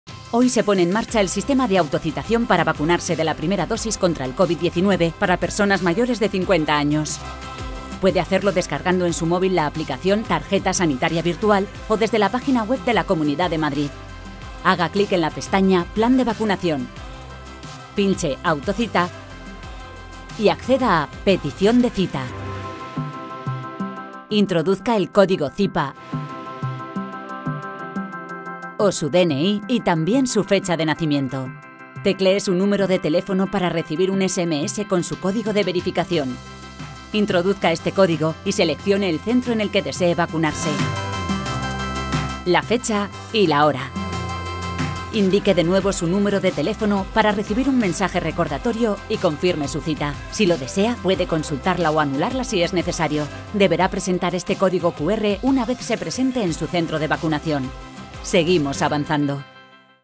Ponemos en marcha el sistema de autocita para vacunación contra el COVID-19 de mayores de 50 años
Ya puedes elegir el día, hora y lugar en el que quieres vacunarte con tu primera dosis. ¡Te explicamos cómo!